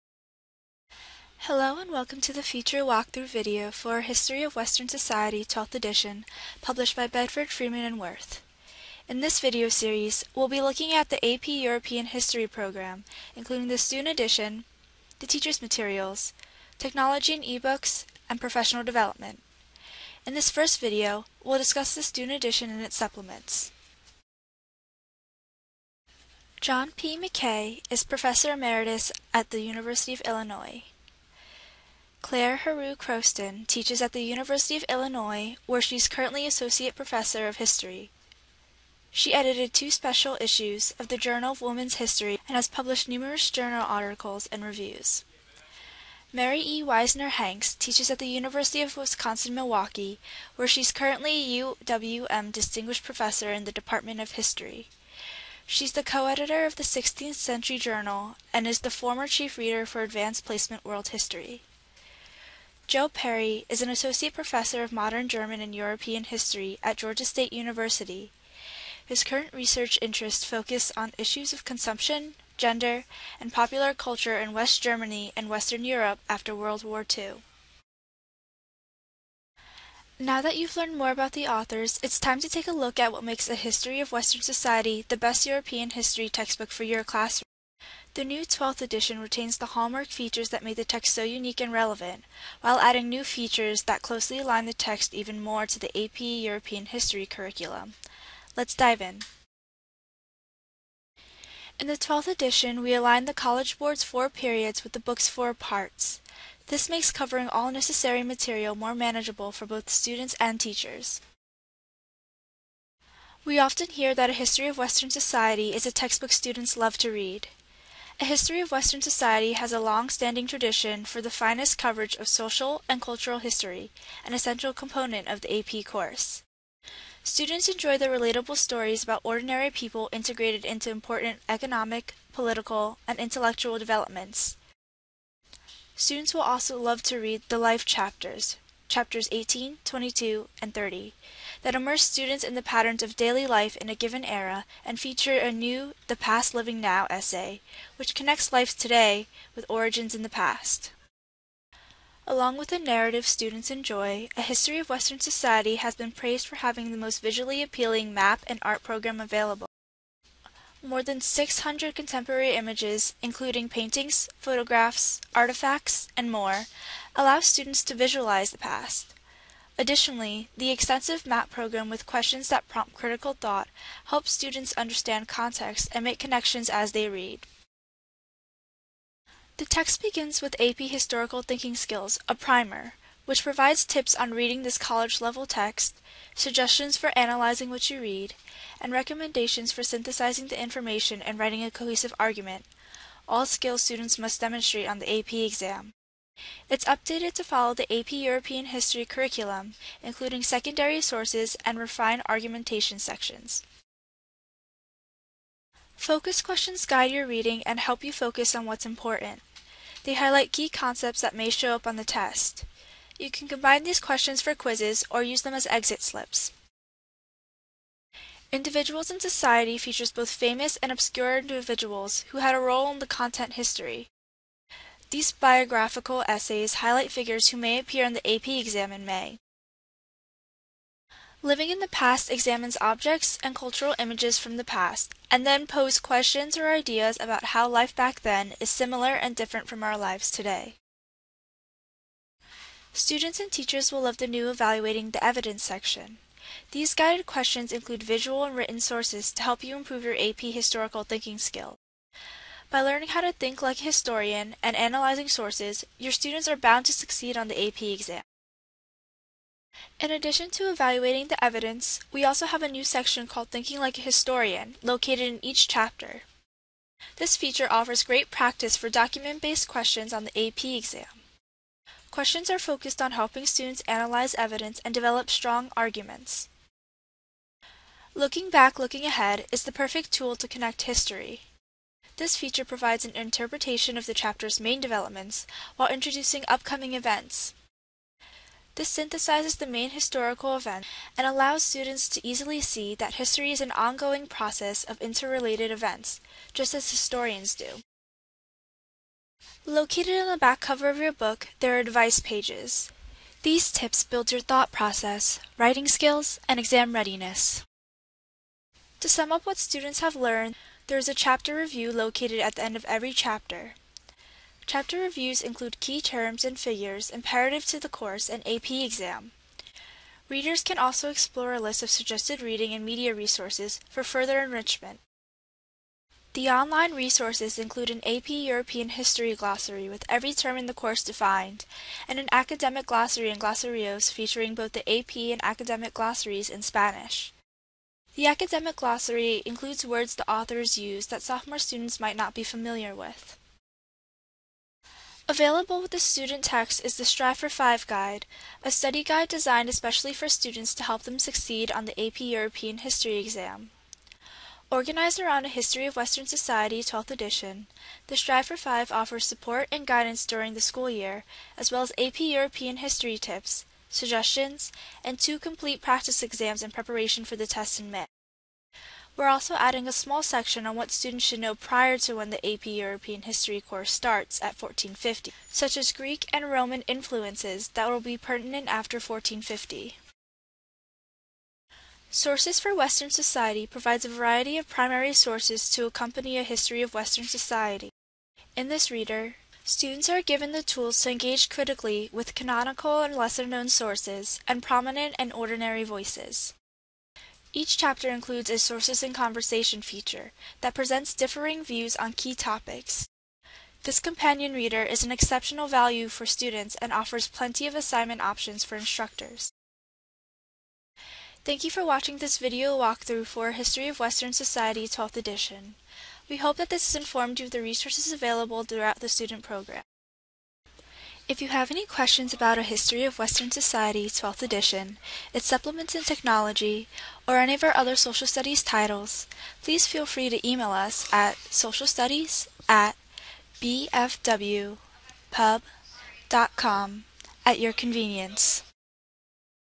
Video Walkthrough for A History of Western Society, 12 Ed. (1/4)
Part 1 of 4 of the Video Walkthrough Series for A History of Western Society, 12th Edition where we discuss the Student Edition.

*AP® is a trademark registered and/or owned by the College Board, which was not involved in the production of, and does not endorse, this product.